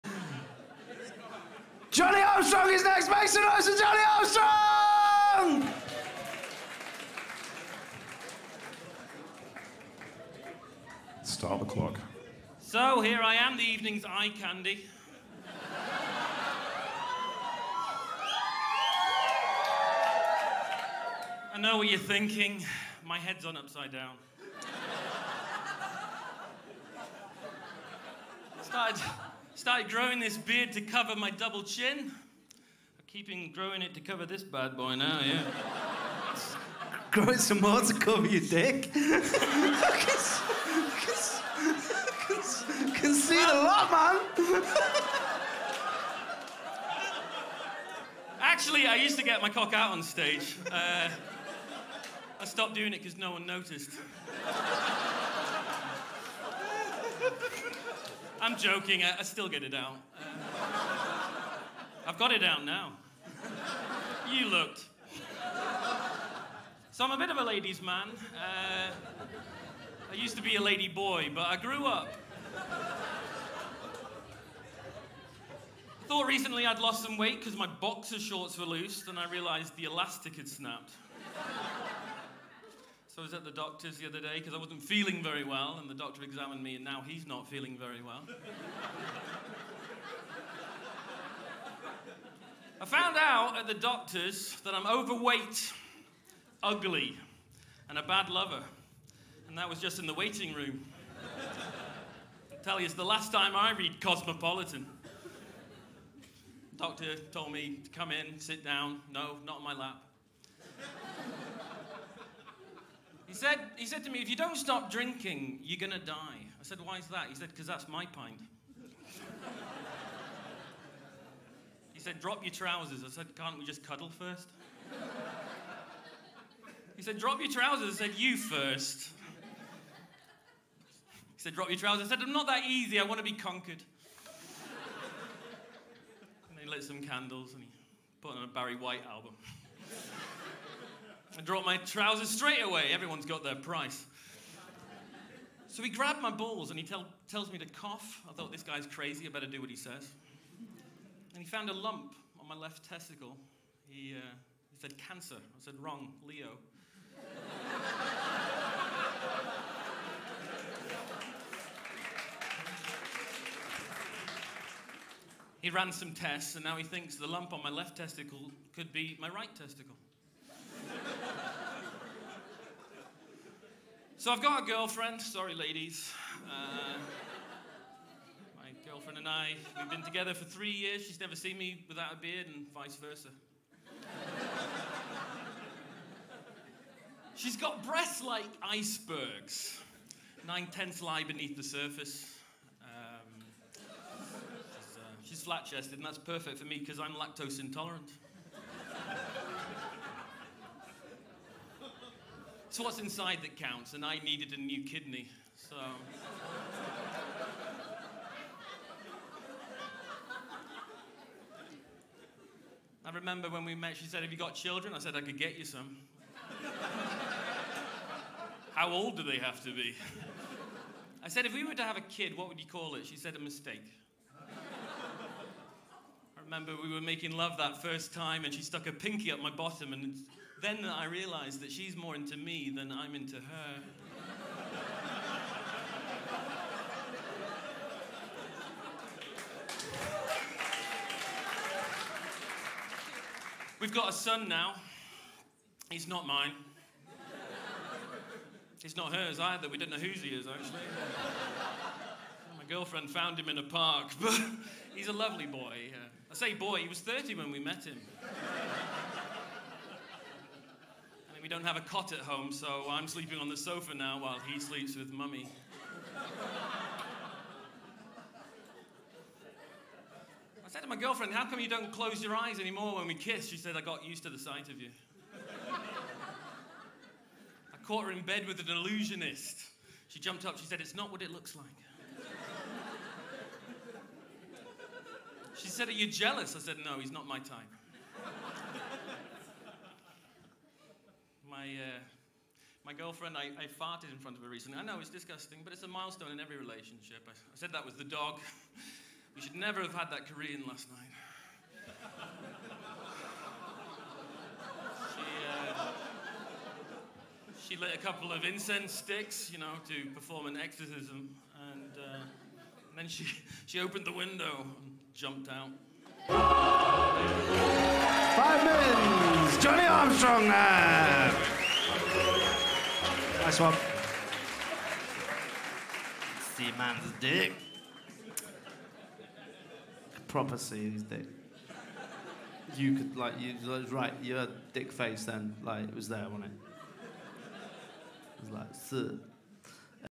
Johnny Armstrong beating King Gong
Proving I can do comedy in England, I survived King Gong at the Manchester Comedy Store, probably the most brutal comedy show in the world. You have to be funny for 5 minutes without getting three cards from the audience. I didn't get any.